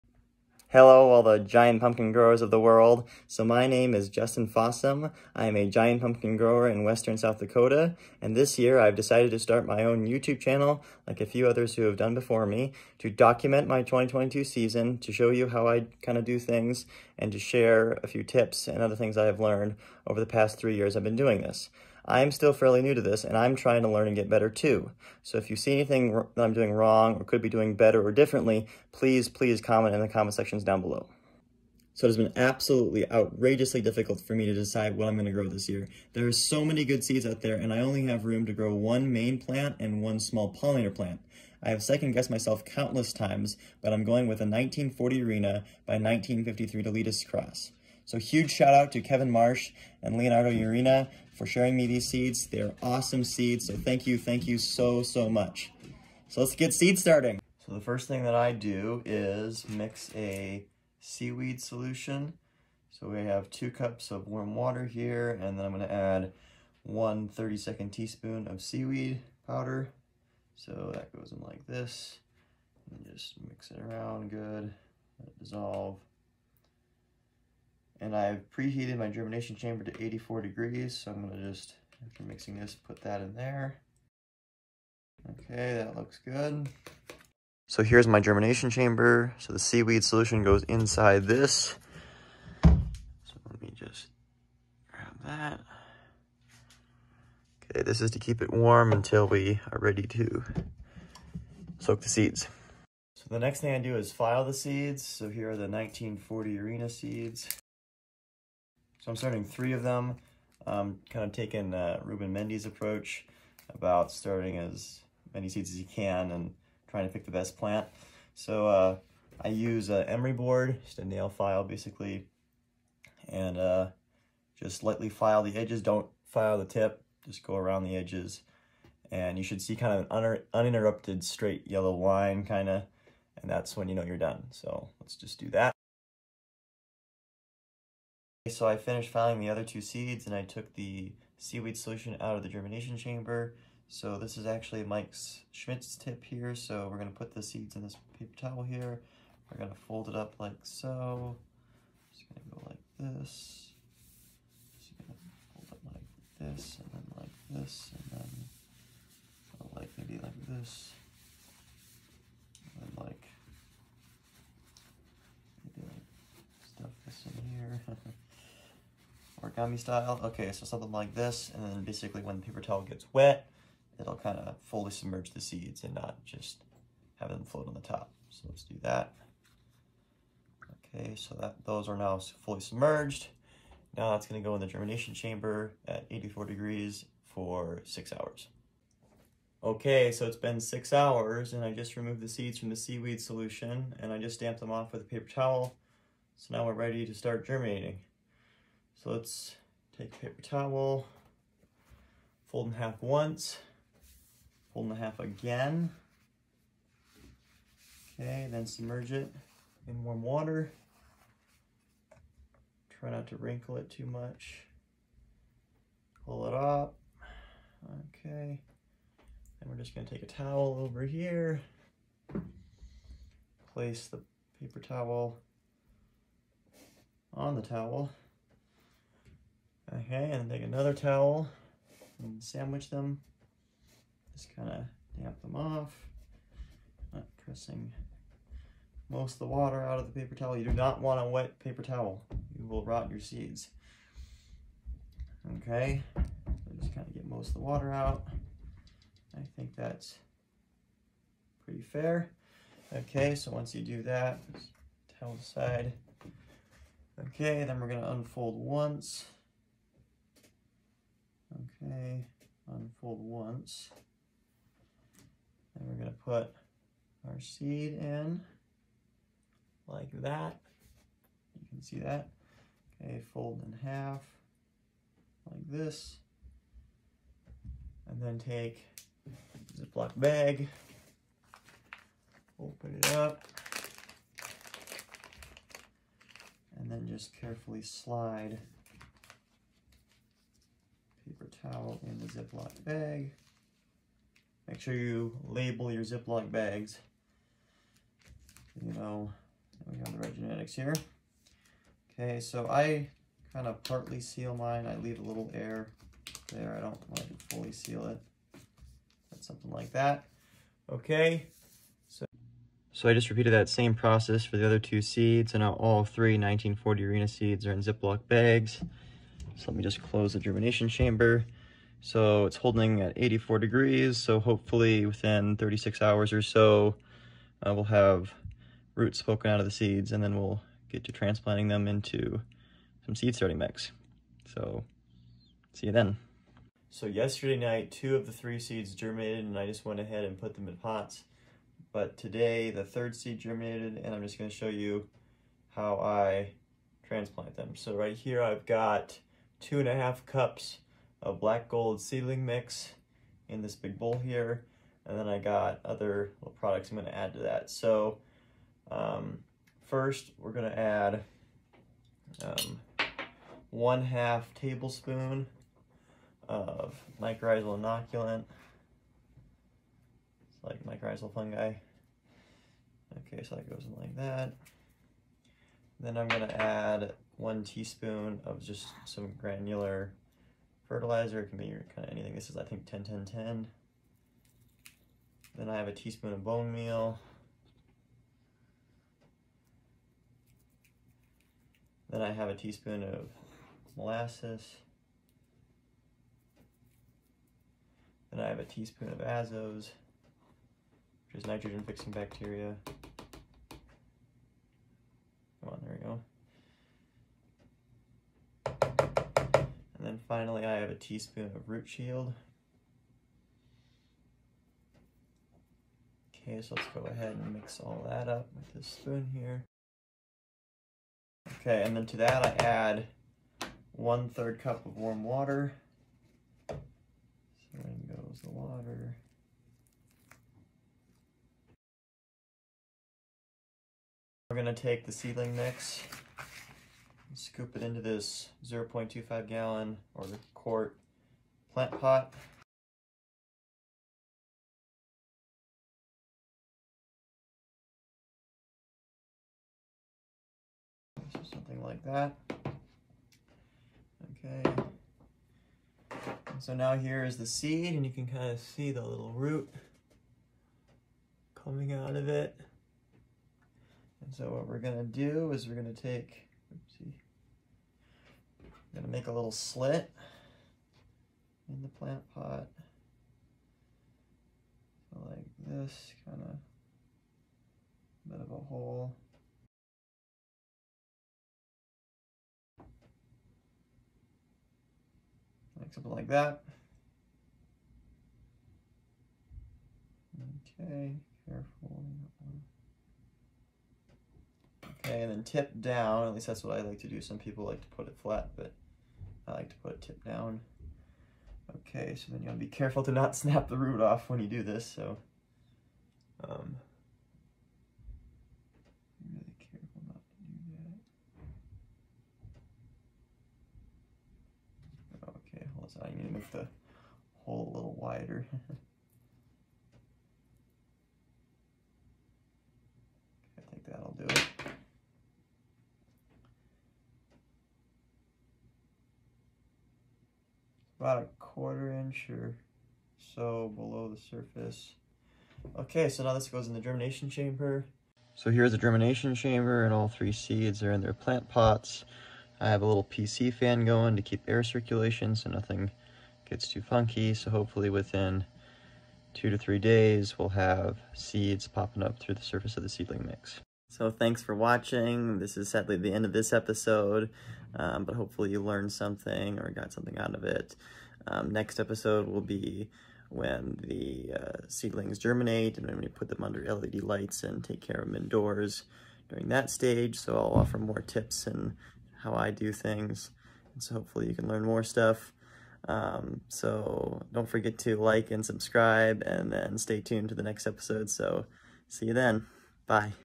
Trying to Grow a 2000 lb Pumpkin Episode 1: How to Start Your Seeds Like a Pro.
It’s seed starting time! Learn how to start your seeds like a pro in this episode.

So below is a list of the things I used in this video:

1. Giant pumpkin seeds. If you are a beginner you can practice with local Giant pumpkin seeds bought from your local garden center. But if you really want to grow a big one (over 1000 pounds) you will need a seed with a pedigree you can find a few there are other places too but these are the main ones that come to mind. You can also contact growers directly and they can send you a few seeds

2. 0.25 Gallon plant pots. Here is a link to get them on Amazon

3. Seaweed powder or liquid. I use this seaweed But I know plenty of people who use Neptune’s Harvest liquid seaweed. (Personally I like the powder cause one it’s more concentrated and so you are not paying for water but also since you store it dry it lasts a lot longer). You might also need some tiny measuring spoons (like 1/32nd teaspoon you can buy these pretty cheaply online)

4. A germination chamber and heat mat with thermostat controller. Now me and my dad built the very nice wooden one you see in the video but they sell cheaper kits so you don’t have to make your own. Or you can use those plastic cake containers.

5. Emory board or nail file. (Fine grit sand paper works too). (You can buy this at Walmart or online although some of you may already have some handy)

6. Mycorrhizal fungi (I use this stuff there are other options too

7. Powdered molasses this is optional but I think it’s a good idea since it provides the sugar to feed the mycorrhizal fungi and other microbes. See this fugue works in a symbiotic relationship with the plants roots. The fungus effectively extends the plants root system by growing fine webs that ships nutrients from afar right to the plant’s “doorstep” and in return the fungus gets a portion of the carbohydrates/sugar the plant is producing. Now by adding the molasses you are kind of tricking the fungus because it thinks that sugar from the molasses is coming from the plant so the fungus helps out the plant without the plant having to sacrifice or give up some of its sugars.
You can buy this online or maybe at some gardening centers it’s called horticulture molasses.

8. Pumpkin Power 9-3-4 slow release granular fertilizer you can find
It’s the second product listed. I am sorry I said 10-10-10 it’s actually this stuff.

9. I used Generic bonemeal (stuff you can find at your local garden center) (I like the down to Earth brand). But what I just heard about to late now for me to have used it but I what you should use instead of the bonemeal is the Gaia Green Power Bloom 2-8-4 slow release granular fertilizer
(apply at the same rate as the bonemeal).

10. If you want to get really serious like me you can add Azos (nitrogen fixing bacteria) found ), and root shield which grows on the plants roots to protect the plant from getting infected with common soil borne pathogens found

11. Miscellaneous stuff: a largish plastic bowl, warm water, stirring spoon, paper towels, 0.25 Gal / 1 quart plastic solid color (not clear since roots like it completely dark) plant pots regular towels, ziplock bags, permanent marker, smaller cups, liquid measuring cups, solid measuring cups, measuring spoons (regular 1 tsp through 1 Tbsp), something to make a slit in the seedling mix for the seed to go into (literally anything will do—the end of a fork or spoon would work).